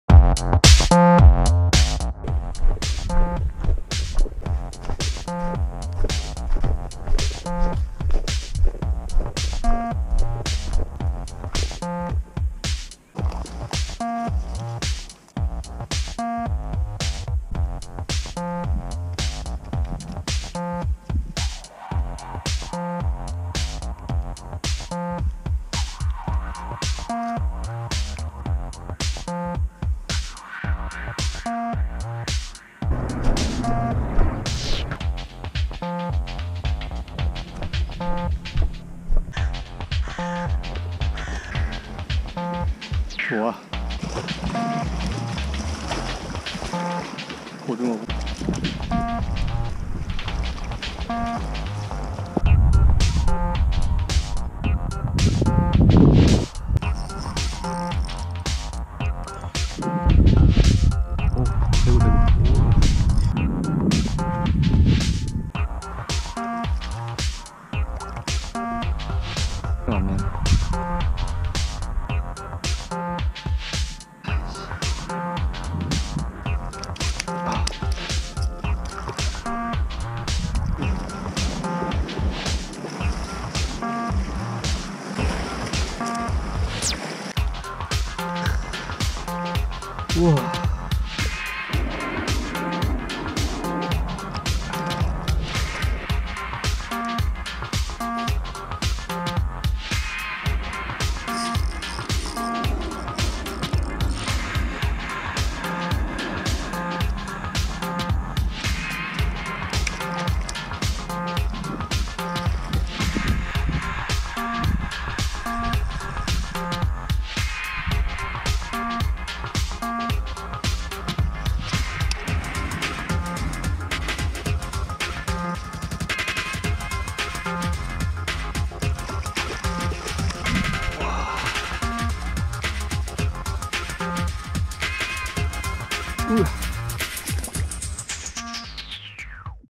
17-20. Fishing in the rain
비가 조금씩 내리는 와중에 오후 짬낚을 나섰습니다.
새로운 포인트에서 마릿수 대구를 만날 수 있었어요.
날씨 좋고 물때 좋은 날 다시 와봐야겠습니다.